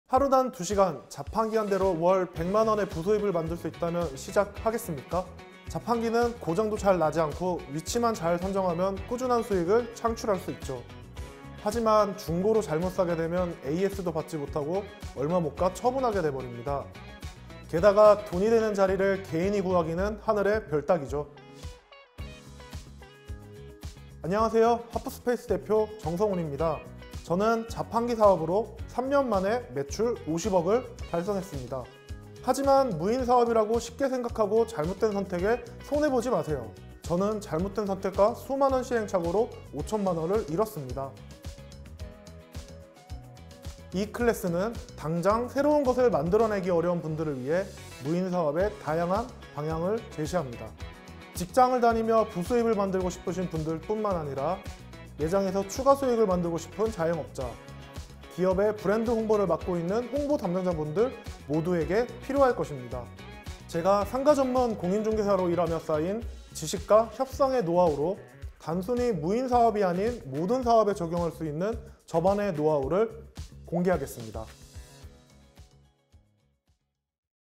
자판기 한 대로 50억 매출 신화🔥 무인 창업으로 돈버는 핵심 노하우
무인 창업 전문가의 50억 매출 달성 노하우가 궁금하다면?